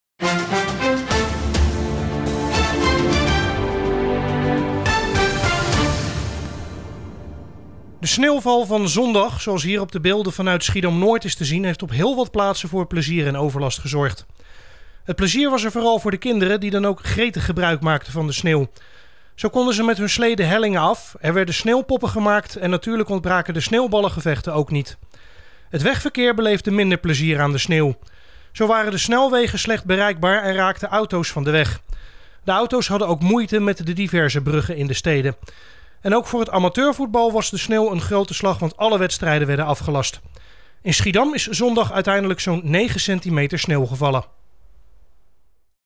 nieuws: Sneeuw zorgt voor plezier en overlast
De sneeuwval van zondag, zoals hier op de beelden vanuit Schiedam-Noord is te zien, heeft op heel wat plaatsen voor plezier en overlast gezorgd. De kinderen maakten gretig gebruik van de sneeuw. Zij konden met hun slee de hellingen af. Er werden sneeuwpoppen gemaakt en natuurlijk ontbraken de sneeuwballengevechten ook niet. He wegverkeer beleefde minder plezier aan de sneeuw. Zo waren de snelwegen slecht bereikbaar en raakte auto's van de weg. De auto's hadden ook moeite met de diverse bruggen in de steden. In Schiedam is zondag uiteindelijk zo'n 9 centimeter sneeuw gevallen.